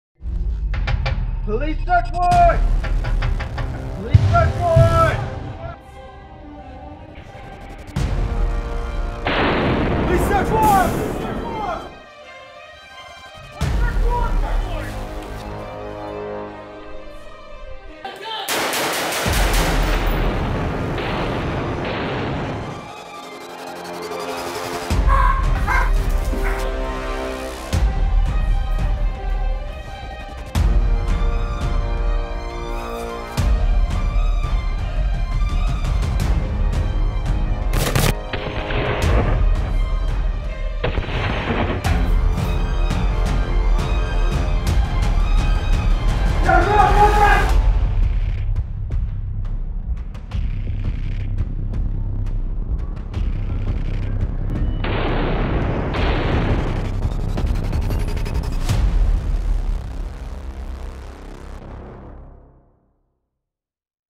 S.W.A.T. Training with the Diamondback Firearms DB15
When danger awaits on the other side of the door, make sure you have the reliable tool needed to get the job done. Get a behind-the-scenes look at this S.W.A.T. training exercise where our DB15 is the team's entrusted weapon of choice.